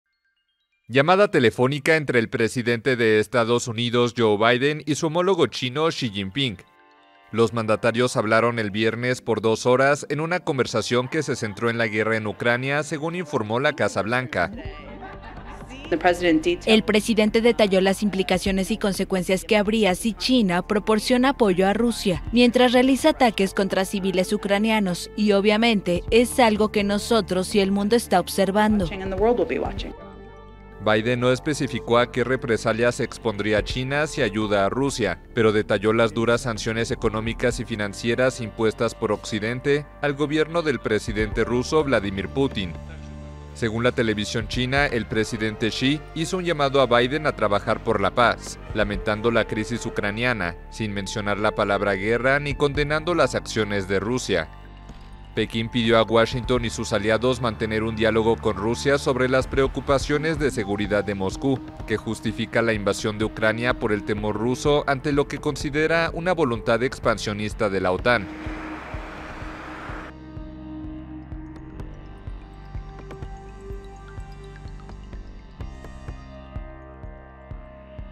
EE.UU. amenaza a XI JINPING las "CONSECUENCIAS" si ayuda a Rusia en la guerra contra UCRANIA
El presidente de Estados Unidos, Joe Biden, avisó  a su homólogo chino, Xi Jinping, de que si ayuda a Rusia en su guerra contra Ucrania tendrá que asumir las "consecuencias", en una conversación en la que Pekín no parece haber dado señales de sumarse a la condena a la invasión.